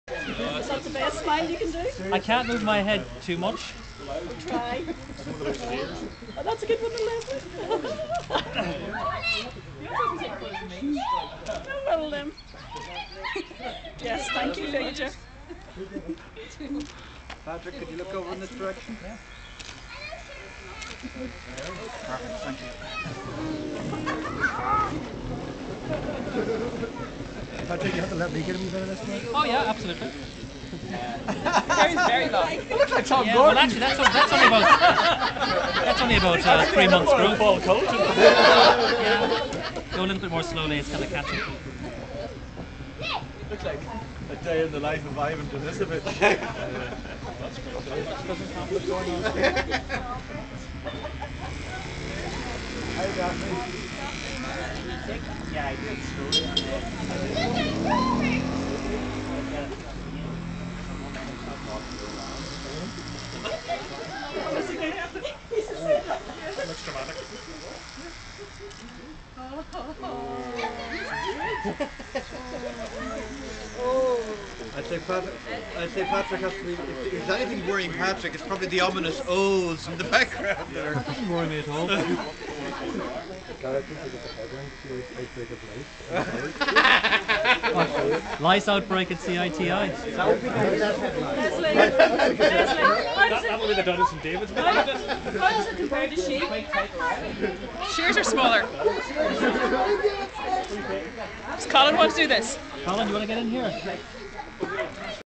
CITI Sponsored Head Shave video 4
Patrick Burke having his head shaved by Lesley Trenier at the Church of Ireland Theological Institute in Dublin on Shrove Tuesday 2009. Patrick organised the event to raise funds for USPG / the Hospital of St francis in Zambia. Recorded on 24 February 2009.